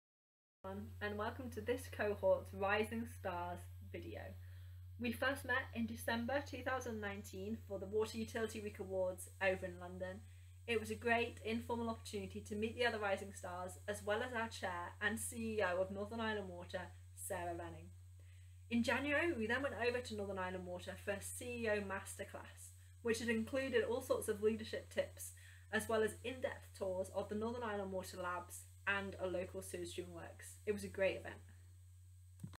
The first events with the Rising Stars
A quick hello to introduce myself as the Institute of Water Rising Stars for the East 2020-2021